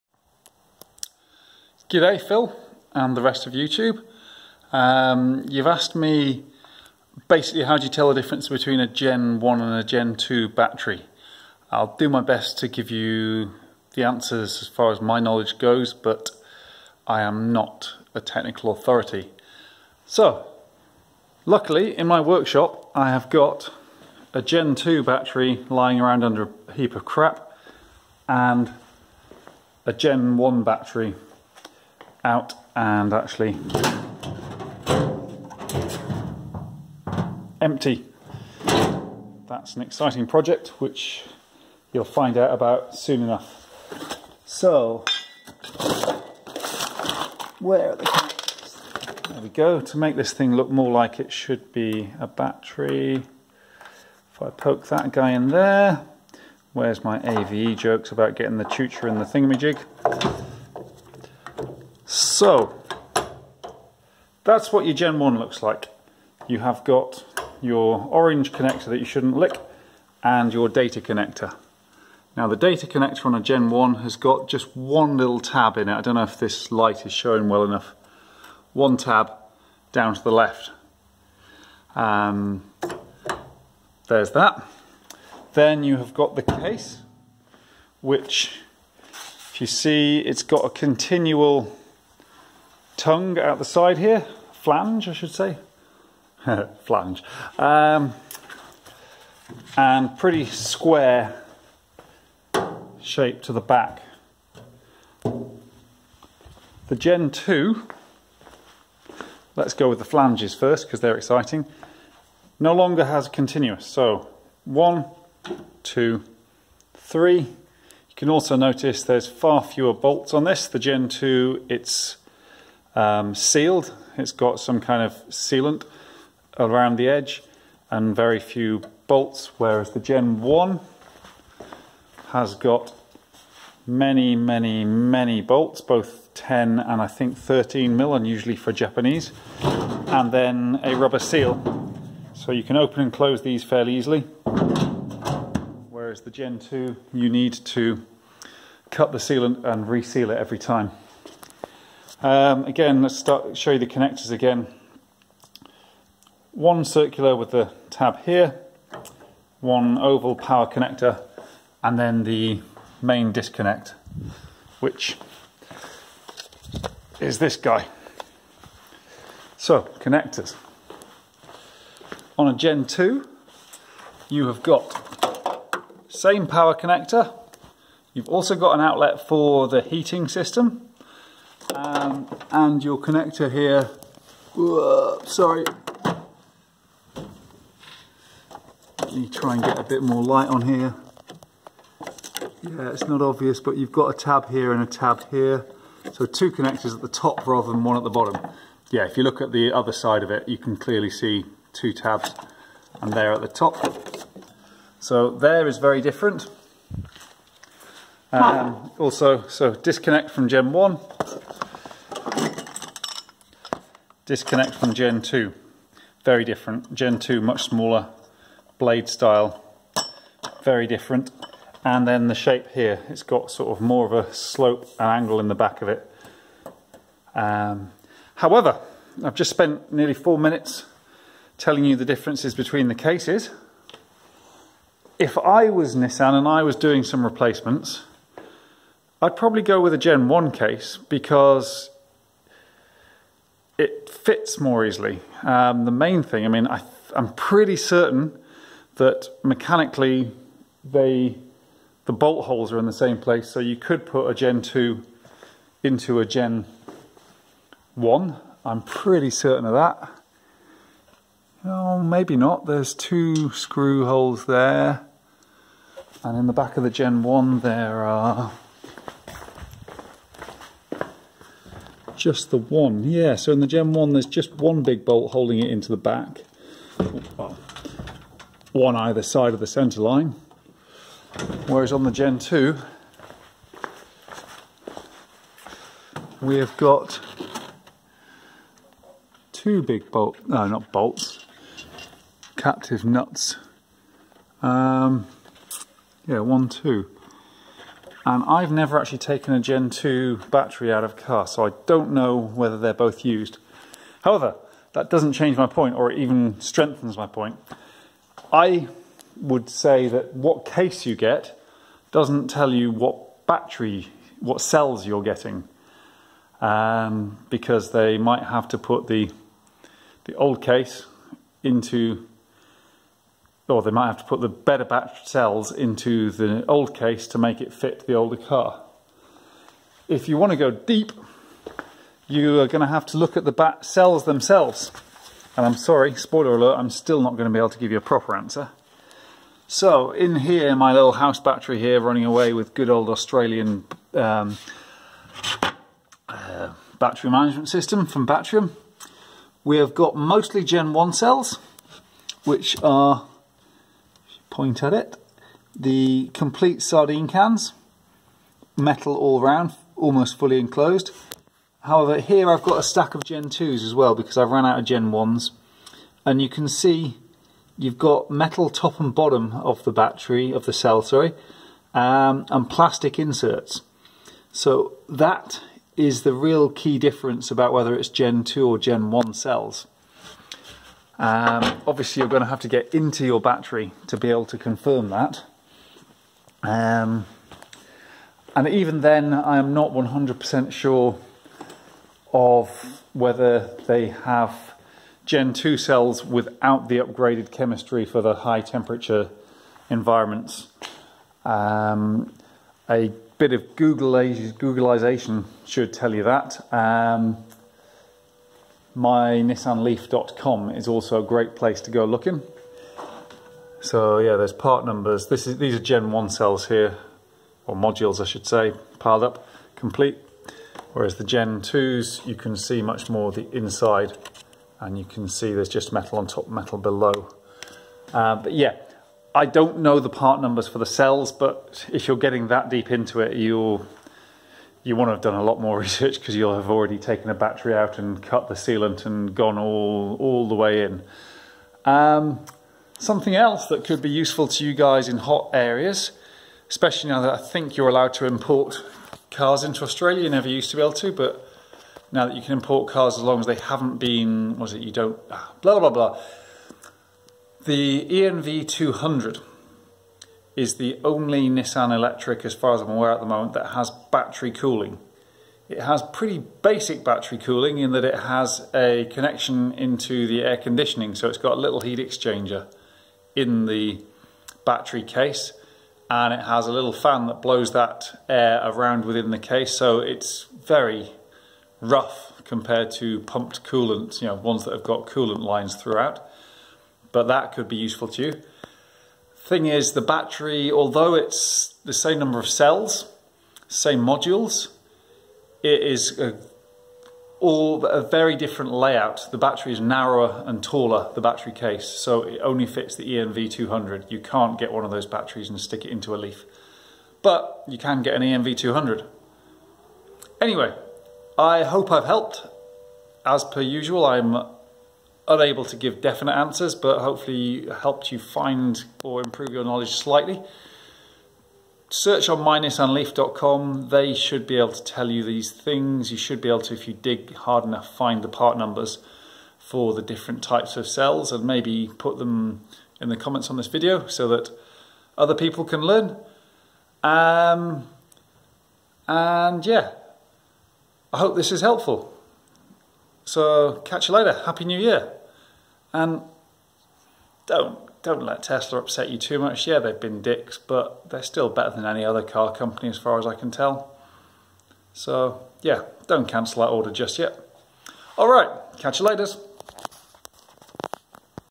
The Differences between G1 & G2 Nissan Leaf Batteries
A man named Phil from somewhere in the hot centre of Australia asked me (on behalf of Mick Dundee I'm sure) how to tell the difference between G1 & G2 Nissan Leaf batteries.
I gave it my best shot but I'm sure I made some mistakes.